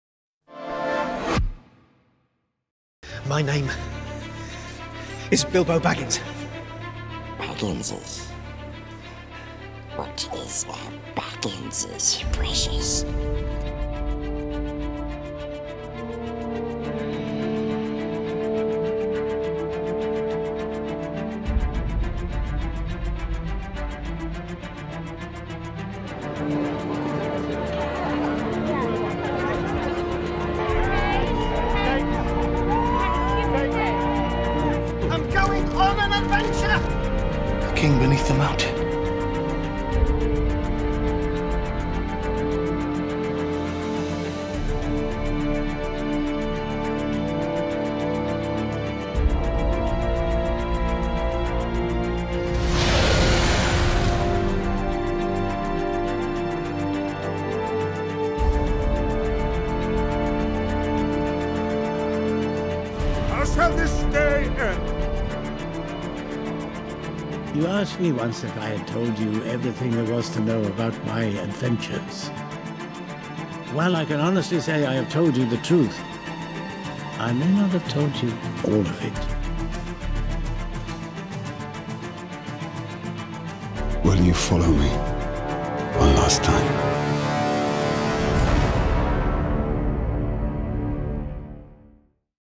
The Hobbit trilogy by numbers
As Peter Jackson prepares to premiere his final instalment in the Hobbit trilogy, The Telegraph explores the numbers behind one of the world's most successful trilogies